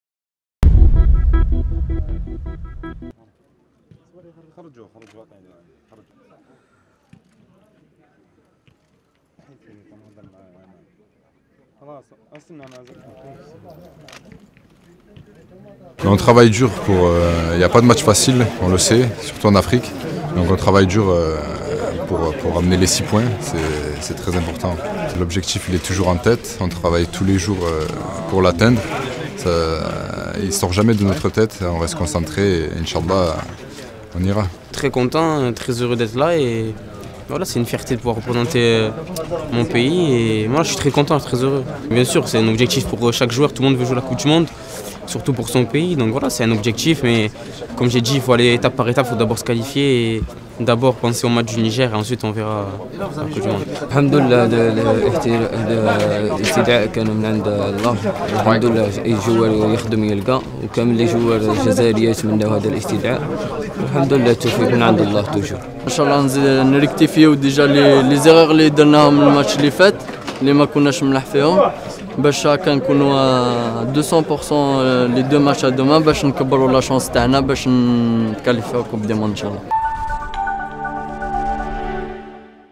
تدريبات المنتخب الوطني إستعدادا لمواجهة النيجر في إطار تصفيات مونديال 2022
الجزائر (وأج) – أجرى المنتخب الوطني لكرة القدم حصة تدريبية إستعدادا للمواجهة المزدوجة أمام النيجر، في إطار  الجولة الثالثة والرابعة من تصفيات كأس العالم 2022 بقطر، المران سبقته، منطقة مختطلة، أجمع من خلالها لاعبو الخضر، بما فيهم المستقدمون الجدد، على ضرورة الفوز في هذه المباراة ذهابا وإيابا، لتعبيد الطريق نحو التأهل إلى المونديال والحفاظ على سلسلة المباريات من دون هزيمة  والتي بلغت 28 مباراة .